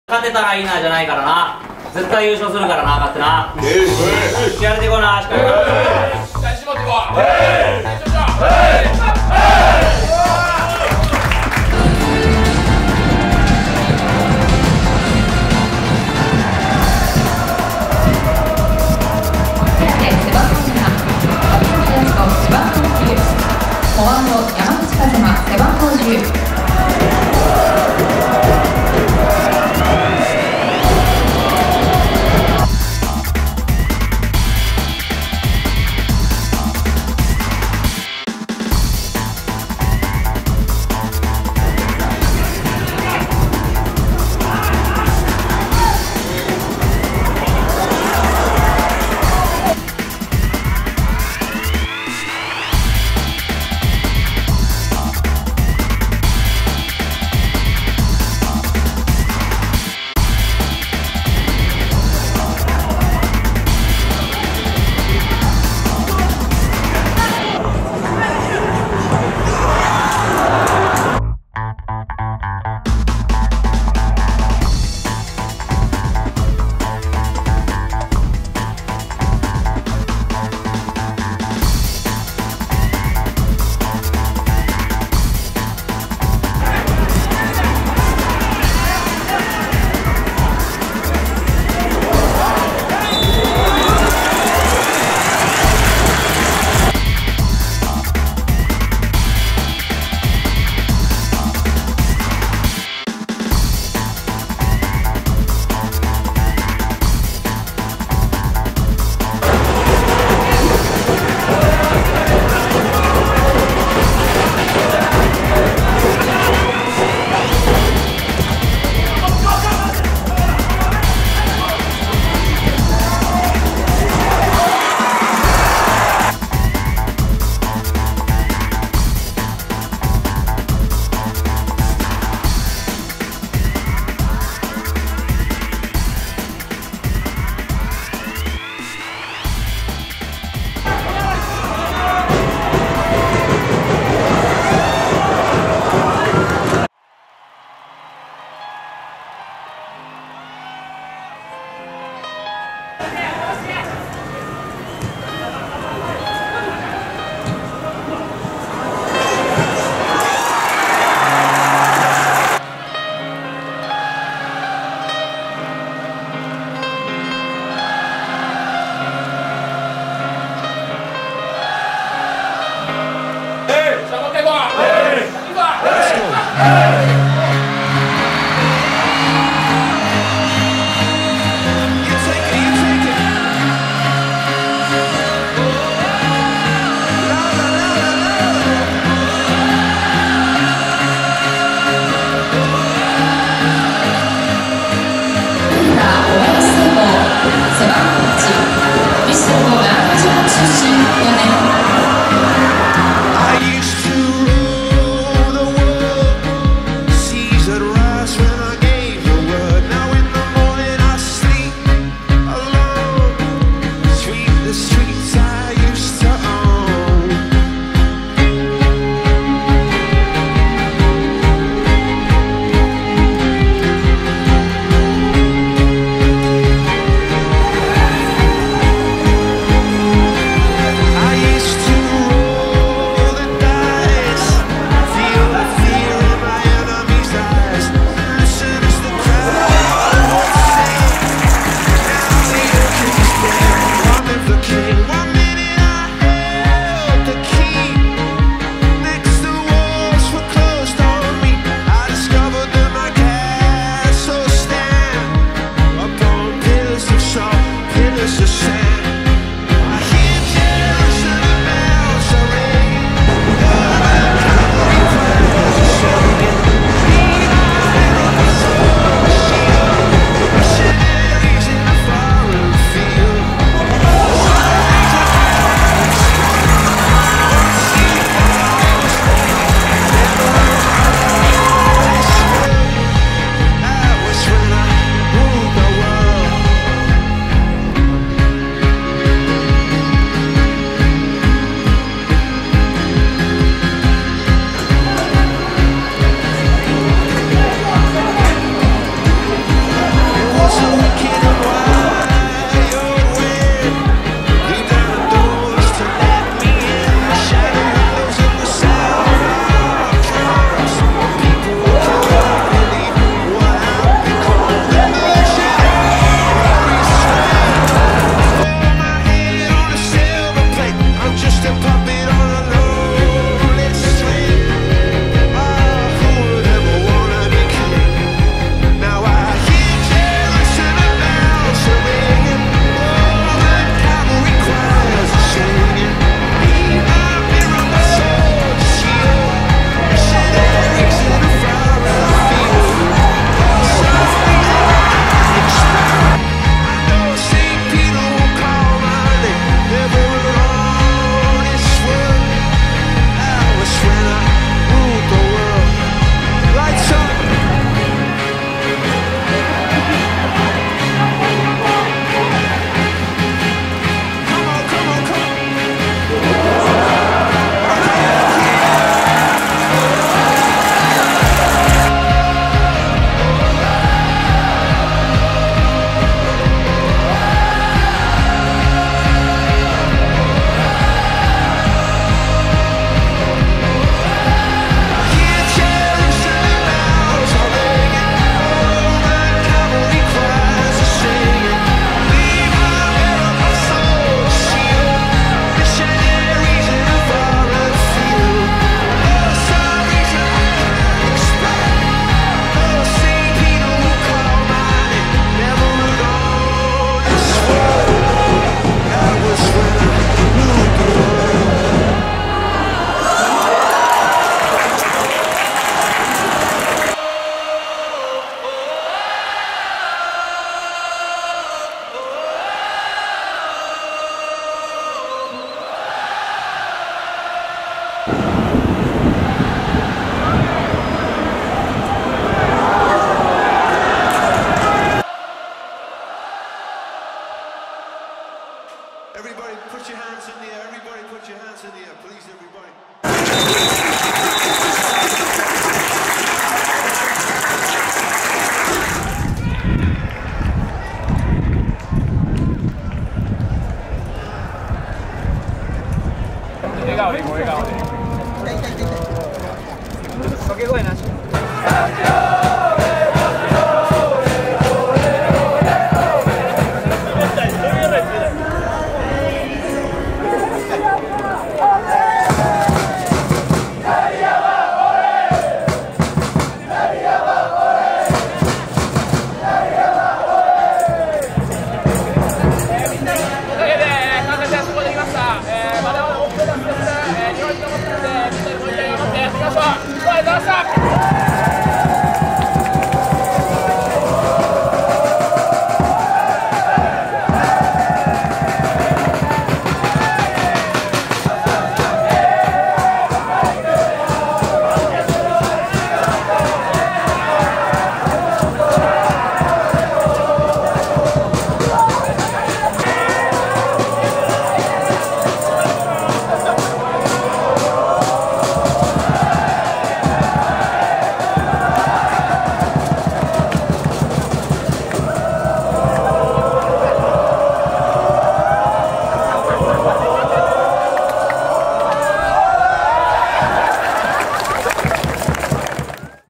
2015/6/6 関西学院大学vs阪南大学 2015関西学生サッカー選手権決勝戦
2015年度第44回関西学生サッカー選手権大会決勝戦
関西学院大学vs阪南大学 5-2 (1-2)
得点者：呉屋大翔(38分、67分、69分、80分、85分)